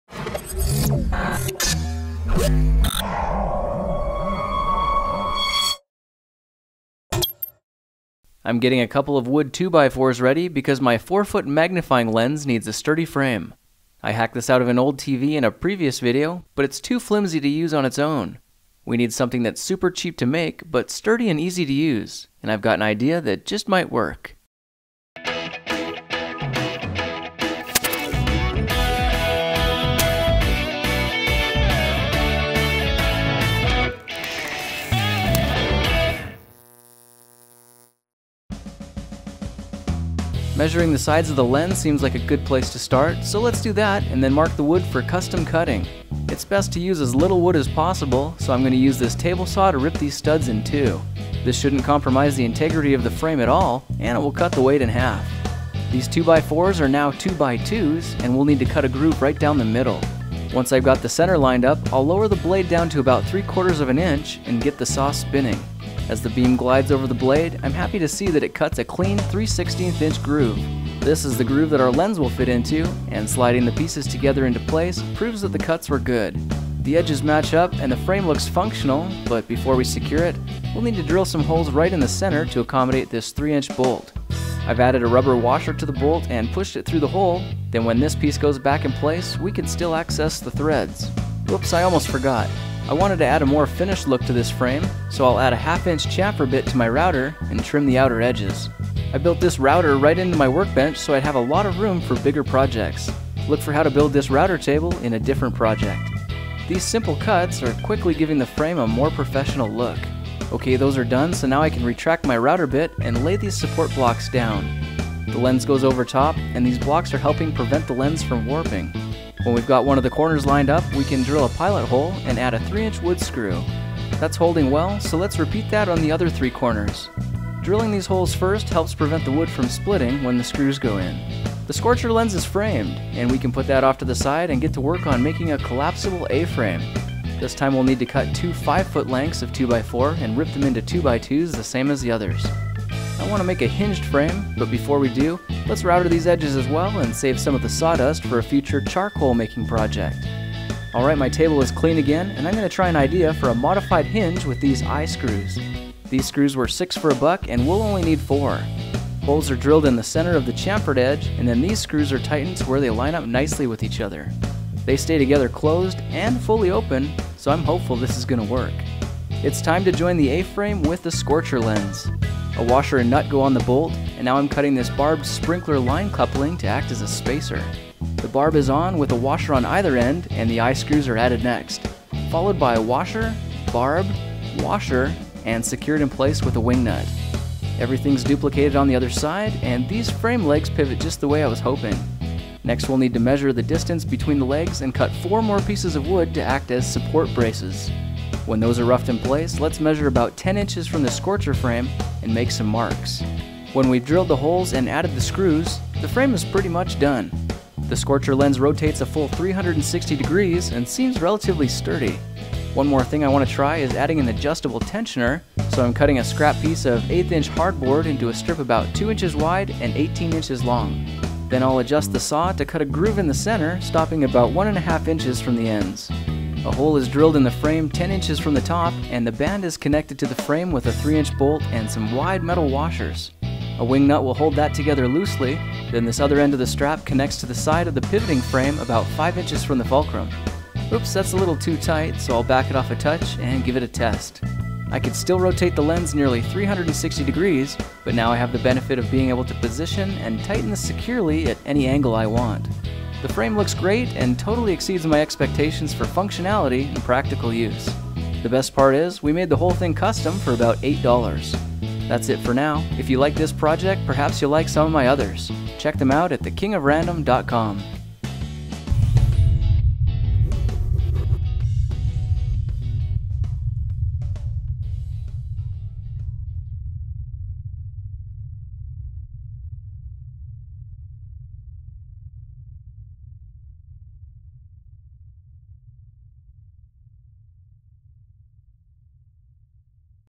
Make a Solar Cooking Frame for Cheap (Fresnel Lens Frame)
Here's a way to frame your Solar Scorcher (Fresnel Lens) in under an hour, and for less than $8!!  This design for a custom "Scorcher Frame" is easy to use, and incredibly cheap to make!

[✓] Small Fresnel Lens

WARNING:

Misuse, or careless use, of tools or projects may result in serious injury or death. Use of this video content is at your own risk.

My 4 foot magnifying lens needed a sturdy frame.  I got this Fresnel lens out of an old TV in a previous video, but it's too flimsy to use on it's own.

I wanted something that's cheap to make, but sturdy and easy to use.  This is an idea I had that seems to work!

The lens can be rotated nearly 360 degrees, but also with the benefit of being able to position, and tighten it securely at any angle you want.

I'm happy with the look, and it totally exceeded my expectations for functionality and practical use.  The best part is the whole thing can be made custom for about $8!